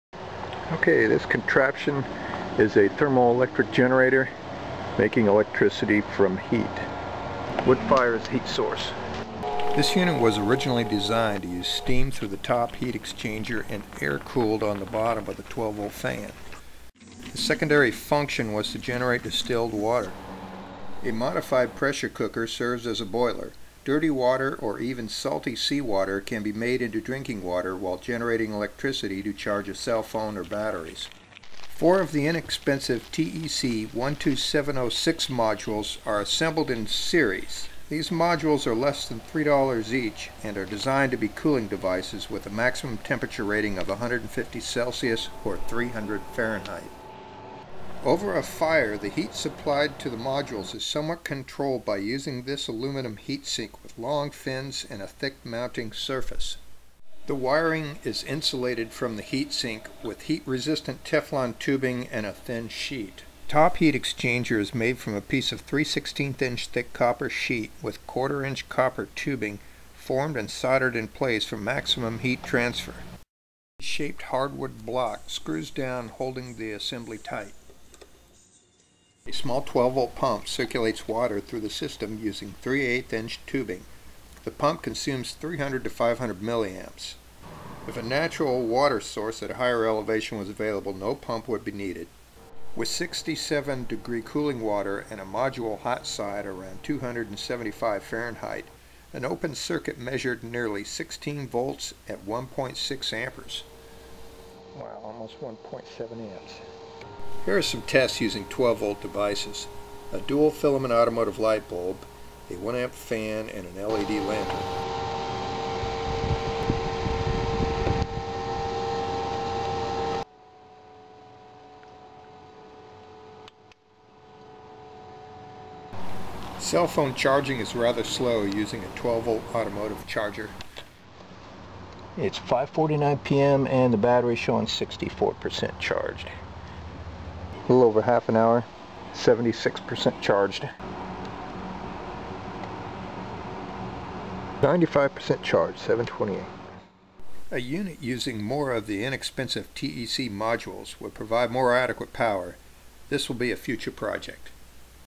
thermoelectric generator using cheap TEC modules
4 cheap Chinese TEC modules assembled in series using wood fire and water cooling generates up to about 8 watts.